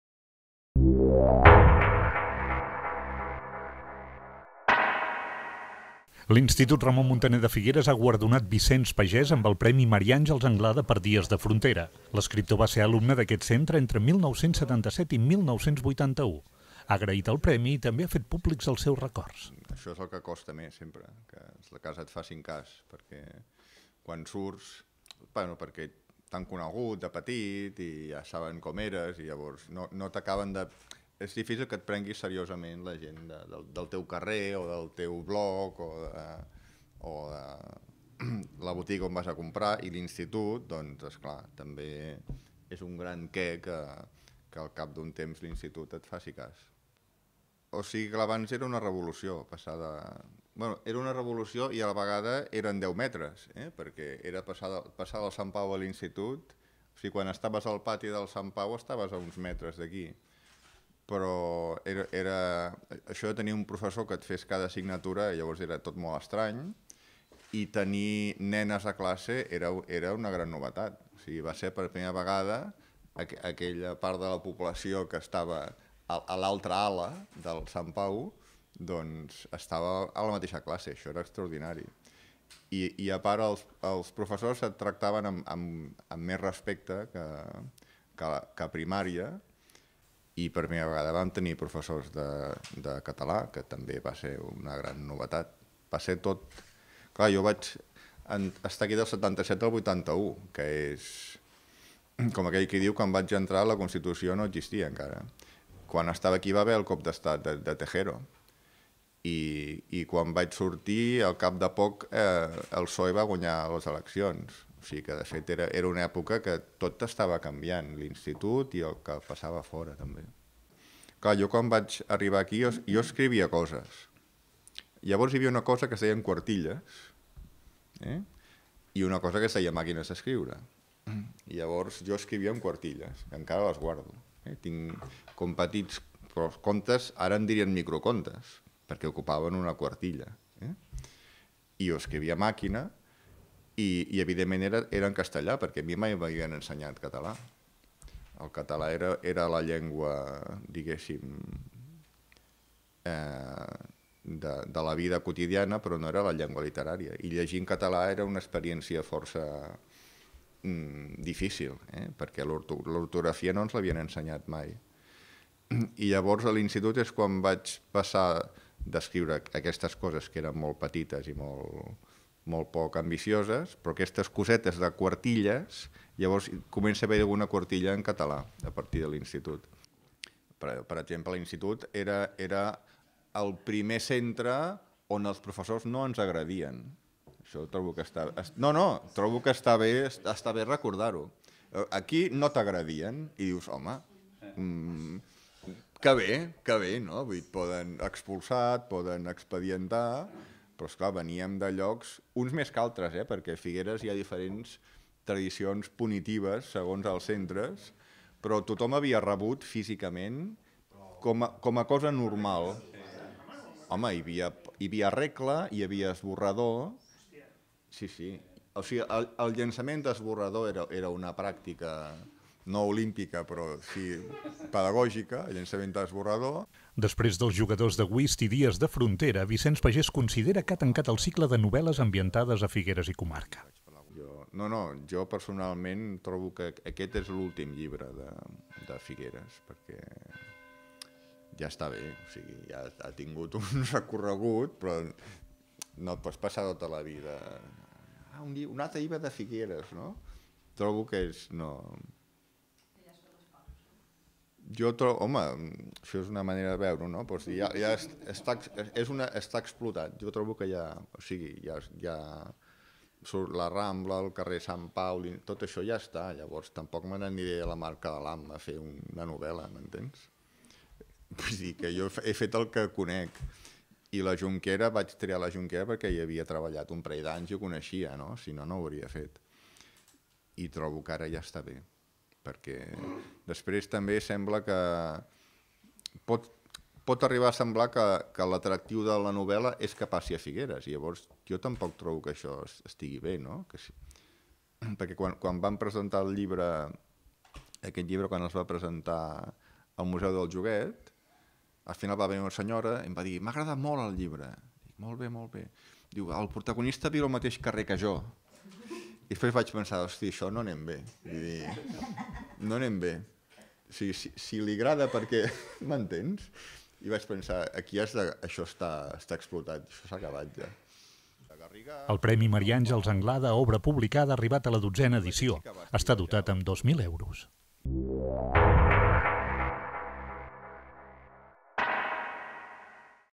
Premi Anglada de l'Institut Ramon Muntaner per a Vicenç Pagès
L'escriptor Vicenç Pagès va ser alumne de l'Institut Ramon Muntaner de Figueres entre 1977 i 1981. Ha estat guardonat amb el premi literari Maria Àngels Anglada per "Dies de frontera".